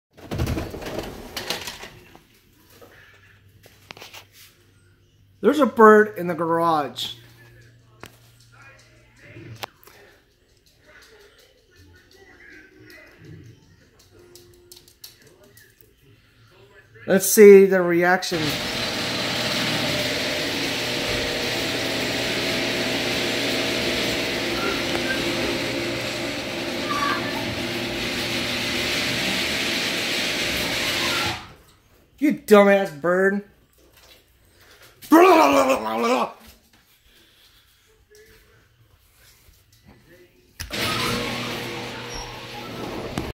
Dumb Bird In Garage
Slider open dumb bird trying to go through window